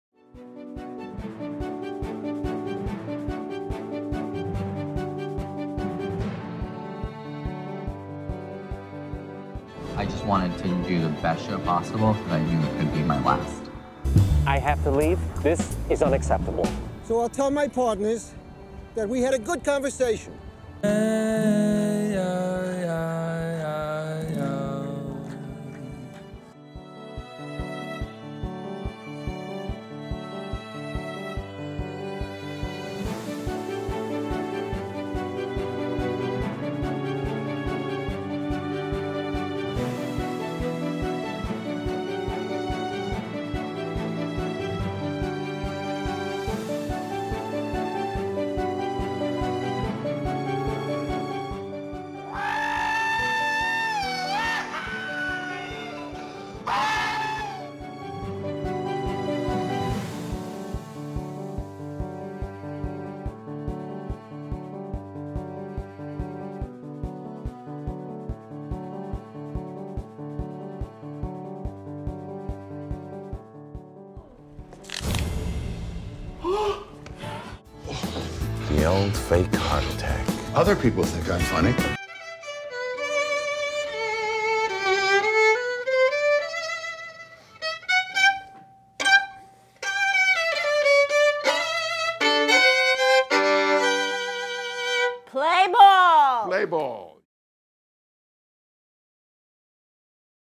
2018 Houston Jewish Film Festival Trailer
Trailer of the Evelyn Rubenstein Jewish Community Center of Houston's Annual Jewish Film Festival.